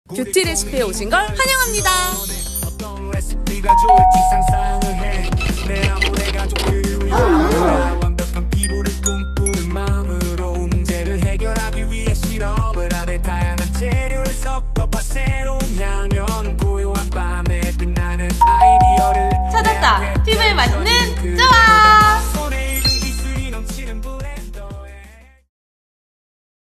[뷰시피] 찾았다 , 피부에 맛있는 조합 !
🔔피부고민 주문서가 들어왔습니다🔔

여러분의 피부고민은 무엇인가요?
피부 타입별로 치부에 맛있는 조합으로 레시피를 만들어드릴게요🍳